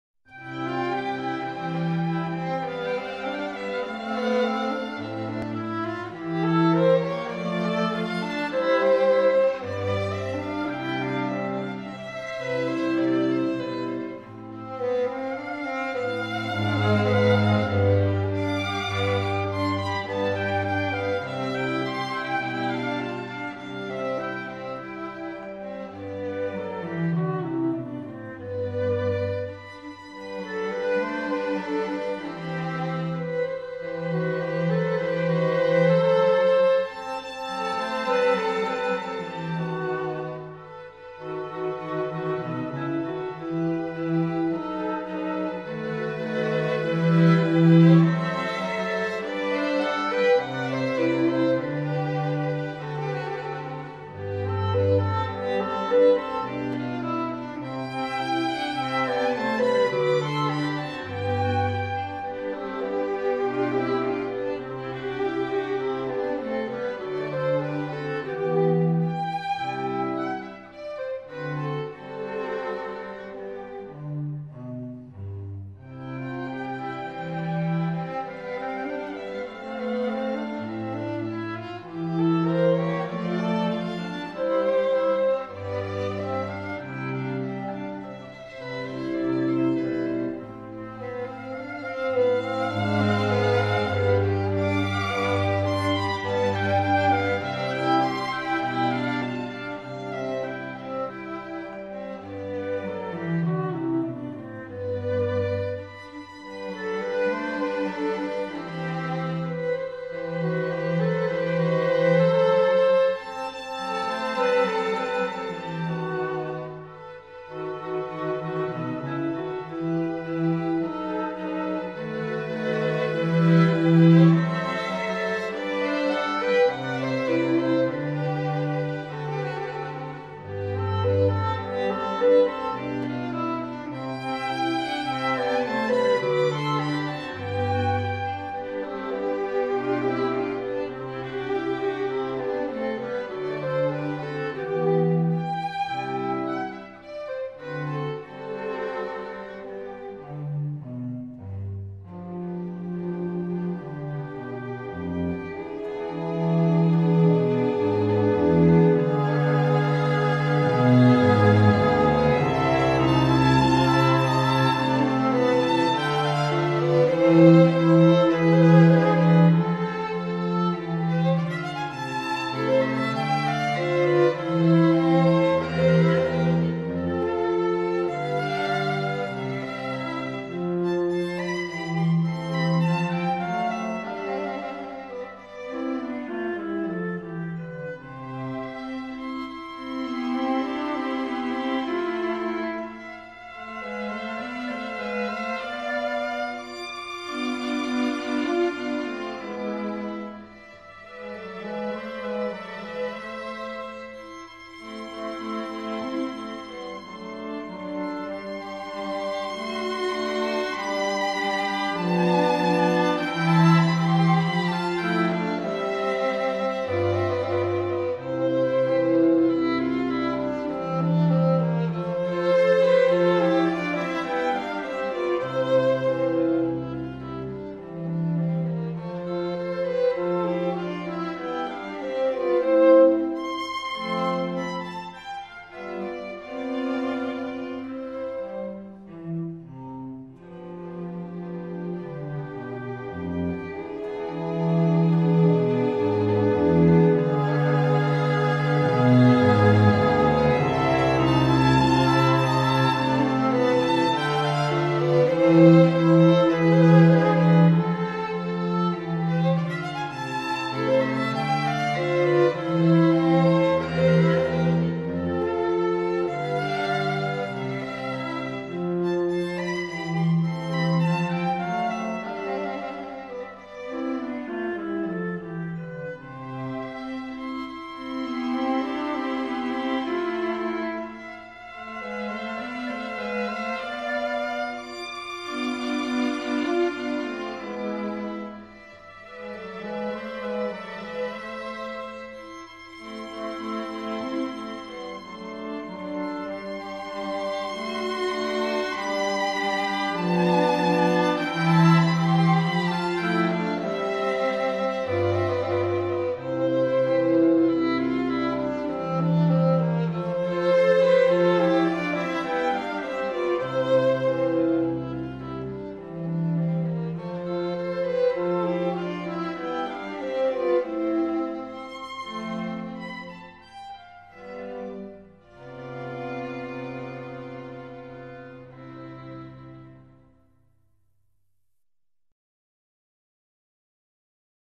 Early Morning Nature in Late Summer 2012
Not far from where I live is a nature preserve called Gilmore Ponds where I like to visit with my camera and assorted lenses. Early in the morning is a magic time to visit. Here are some of the photos taken early on Friday August 24th 2012. The music reflects the serenity and peacefulness of nature. (3.5 mins)